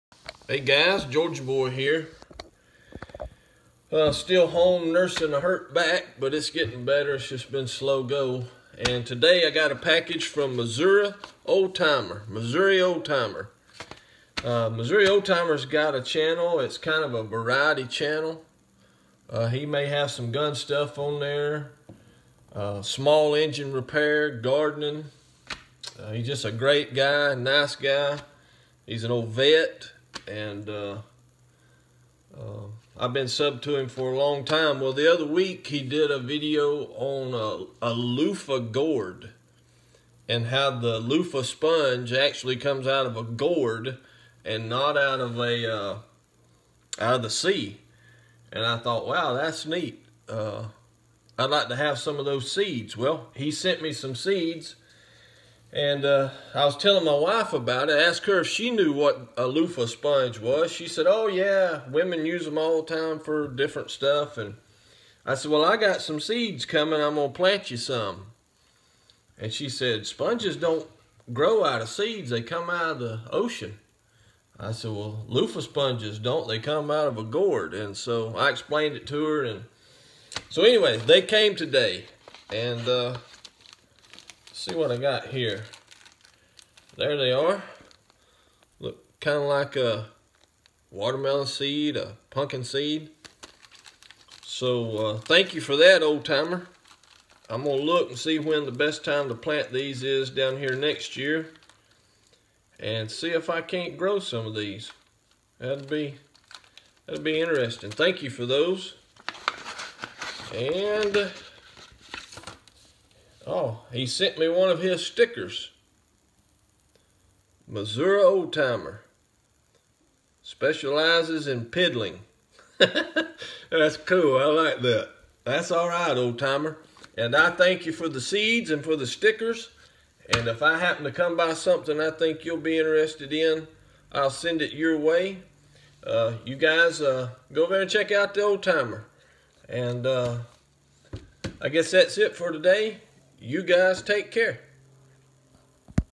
Package from MissouriOldTimer
Got some Luffa gourd seeds from MissouriOldTimer